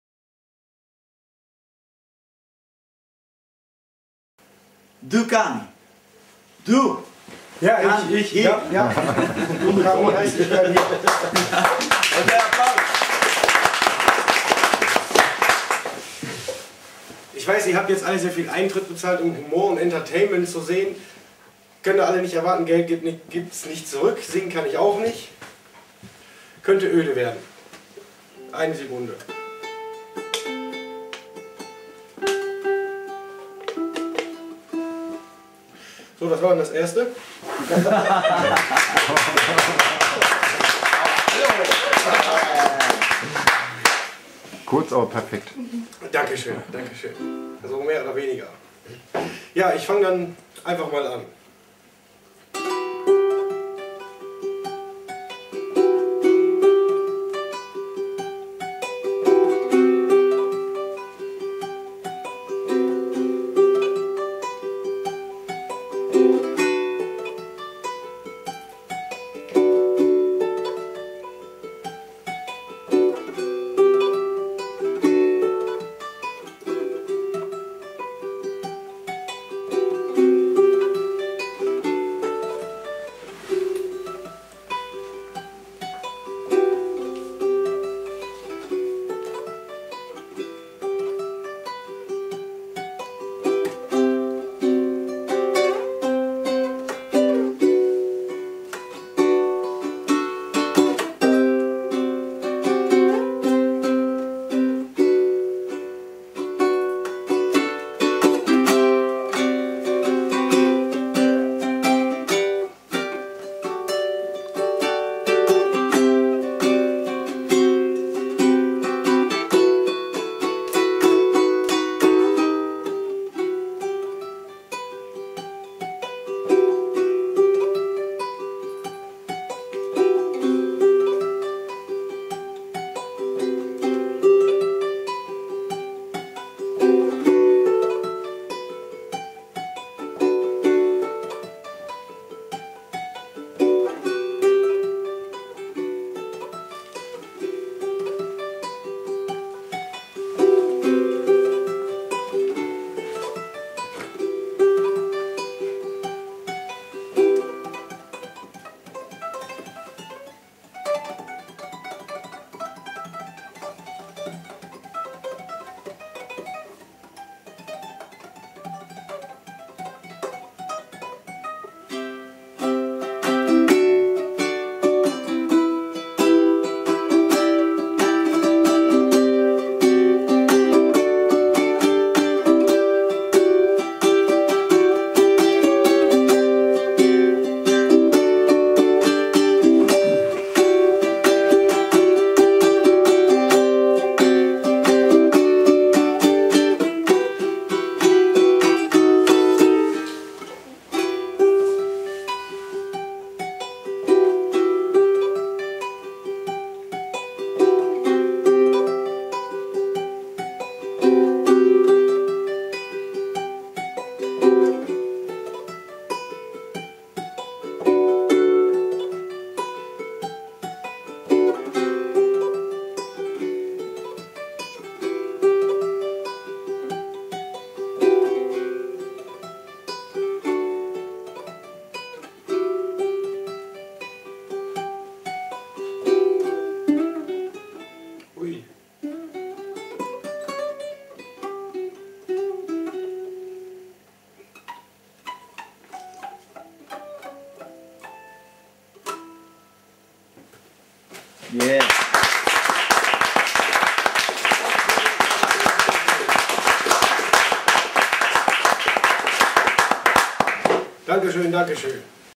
Dukami (1)
Auftritt (1. Lied) von Dukami beim 2. Winterswijker Ukulelen-HotSpot (13. bis 15. Juni 2008.